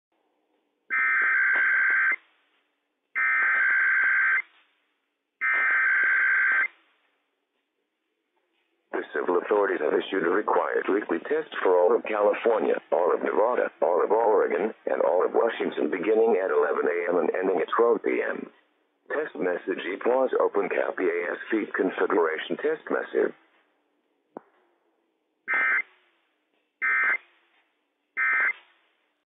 IPAWS Required Weekly Test - WWF56 - EAS #60 - 11:01 AM PST 3/1/21
Required Weekly Test issued by the Civil Authorities at 11:01 AM PST 3/1/21 for All of CA, NV, OR, and WA.